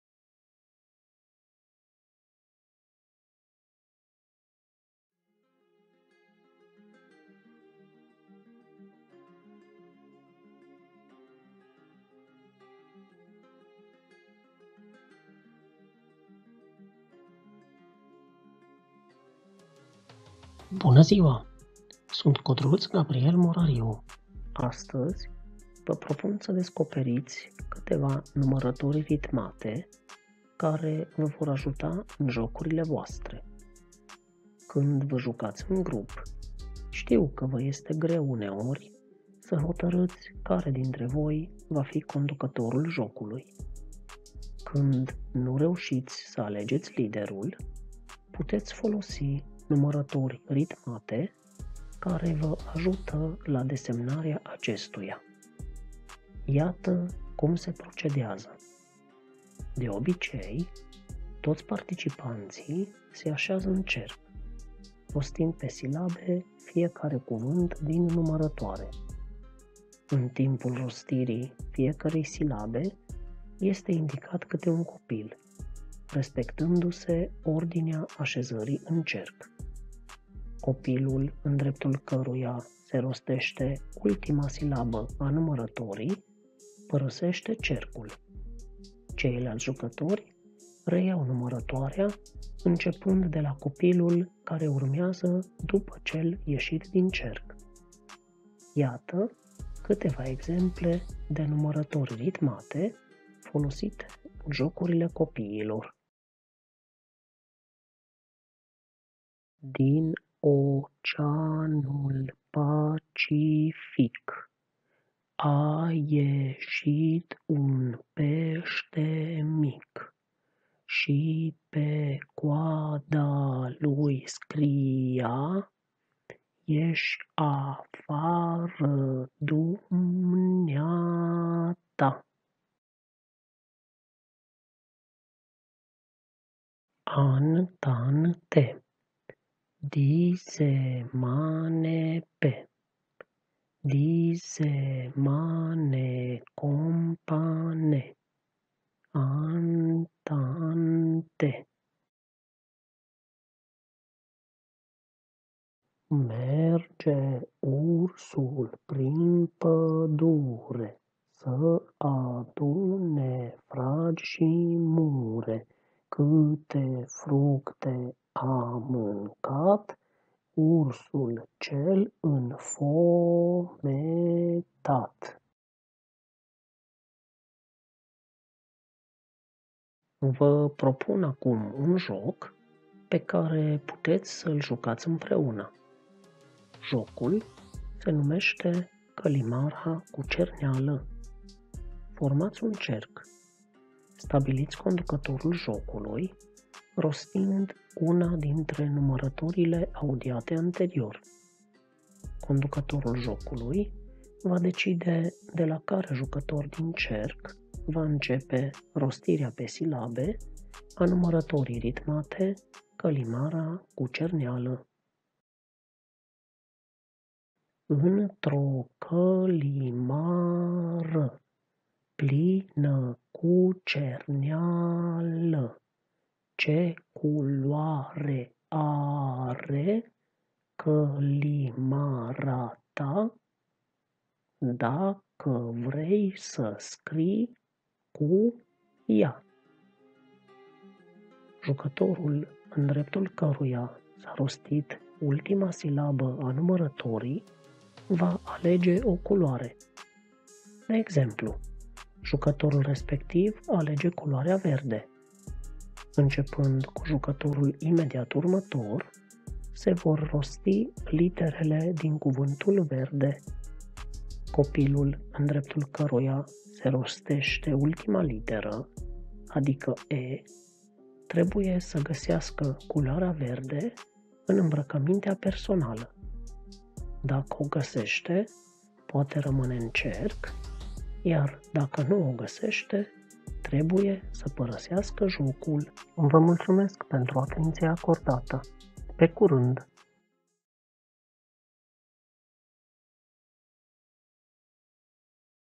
Numărători ritmate
La începutul resursei sunt prezentate câteva exemple de numărători ritmate folosite în jocurile copiilor:
La finalul resursei, se propune jocul  Călimara cu cerneală.
Conducătorul jocului va decide jucătorul din cerc de la care va începe să rostească pe silabe numărătoarea ritmată, în sensul acelor de ceasornic.
Jucătorul în dreptul căruia s-a rostit ultima silabă a numărătorii va alege o culoare. Începând cu jucătorul imediat următor se vor rosti literele culorii alese. Copilul în dreptul căruia se rostește ultima literă a culorii trebuie să găsească culoarea respectivă în îmbrăcămintea personală. Dacă o găsește, poate rămâne în joc, iar dacă nu o găsește, trebuie să părăsească cercul.

Autor: Codrut Gabriel Morariu